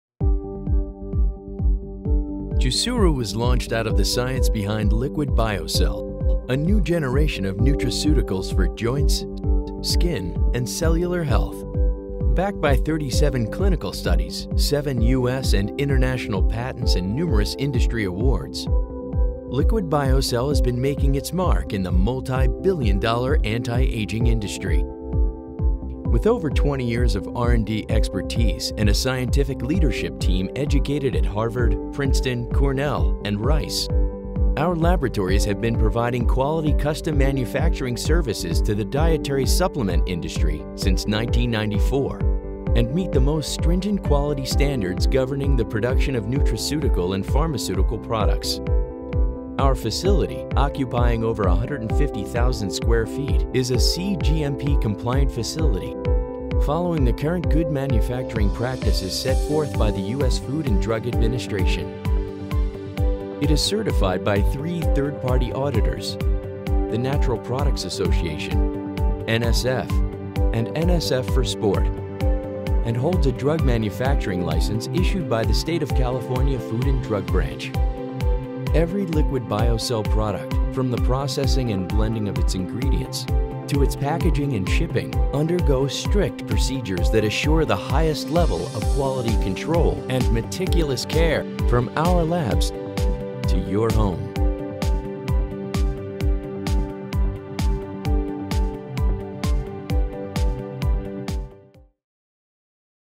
Jusuru Quality Manufacturing
With over 20 years of R&D expertise and a scientific leadership team educated at Harvard, Princeton, Cornell, and Rice, our laboratories have been providing quality custom manufacturing services to the dietary supplement and homeopathic drug industry since 1994. In an over 150,000 sq. ft facility that is NSF and NSF for Sport Certified, Jusuru products meet the most stringent criteria of cGMP compliance in manufacturing practices.